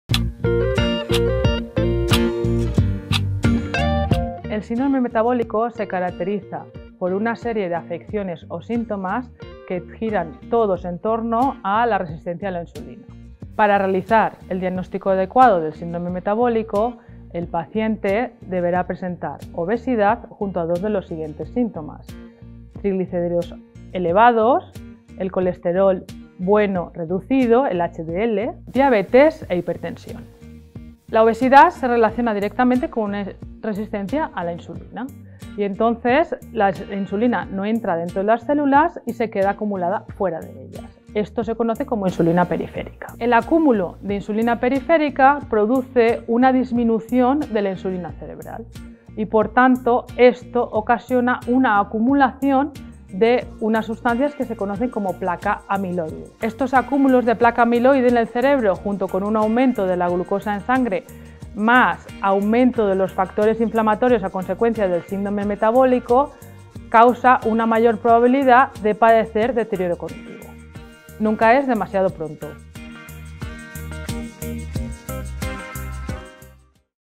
Síndrome Metabólico | De la A a la Z, factores de riesgo modificables en la enfermedad de ALZHEIMER
De la A a la Z, factores de riesgo modificables en la enfermedad de Alzheimer. Hablamos de Síndrome metabólico y deterioro cognitivo con la profesora Dra. Mariam López, vicedecana de Medicina de la Universidad CEU Cardenal Herrera y miembro de la Cátedra para el estudio del Deterioro Cognitivo MICOF – CEU UCH.